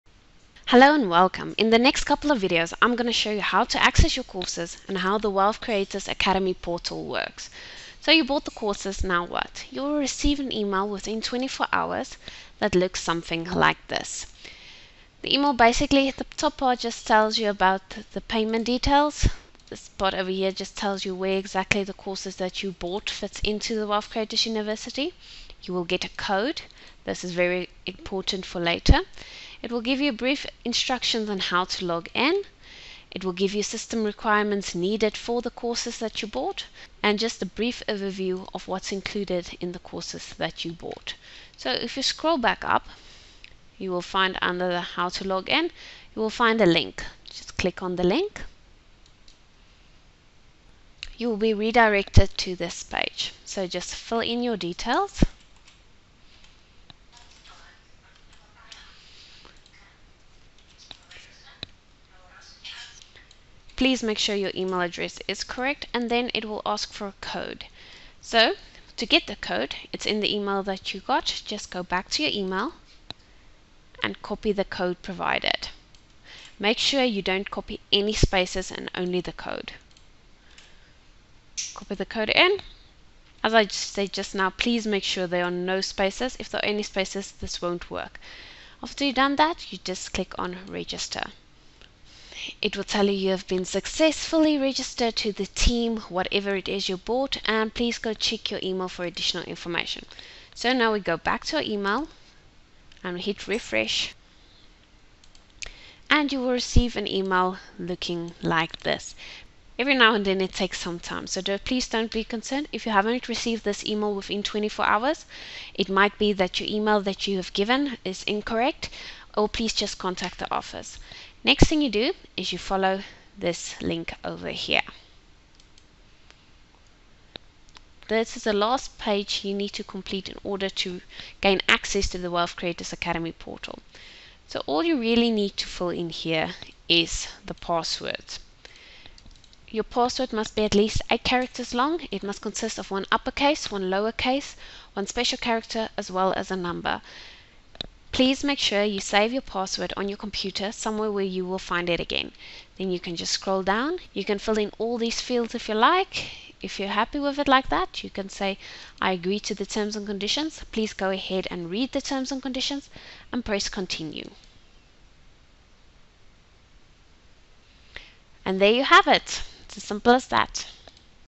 Access Courses
How to access your courses in the Wealth Creators Academy Portal.